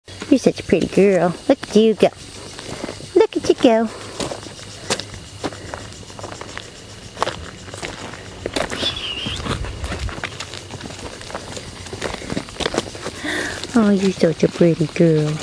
Embers Miss Pink Cashmere Black Female Puppy
Embers Miss Pink Cashmere is a black female puppy. She is so sweet and loves to get lots of attention. She is just adorable.